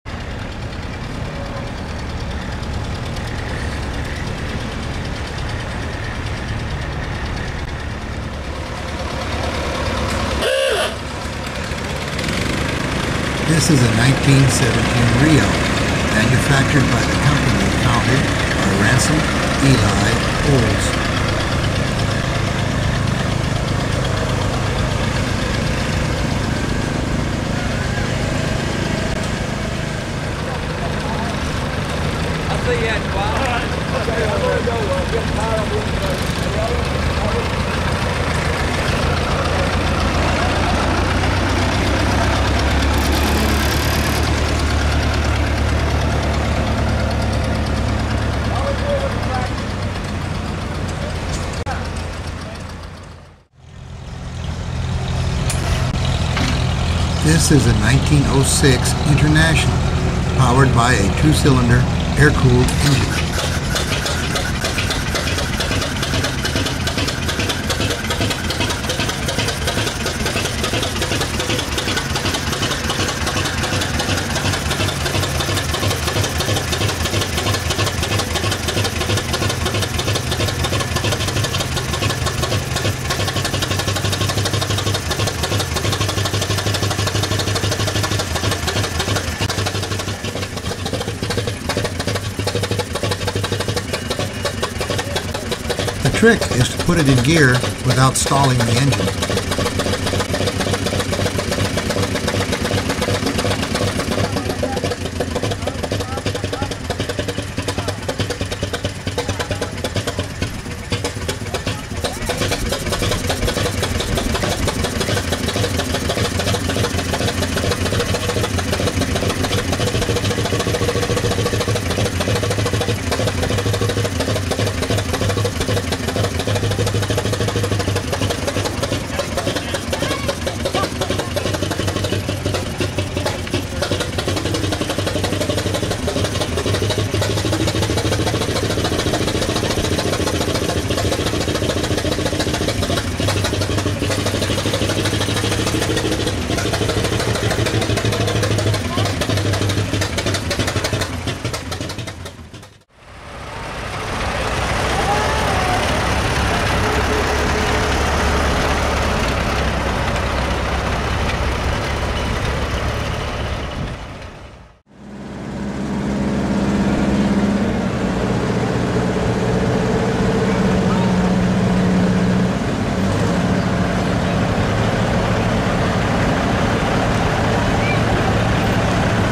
Trucks at TEDTEA
Four antique trucks driving on the show grounds at the Texas Early Day Tractor and Engine Association show in Temple TX in October, 2009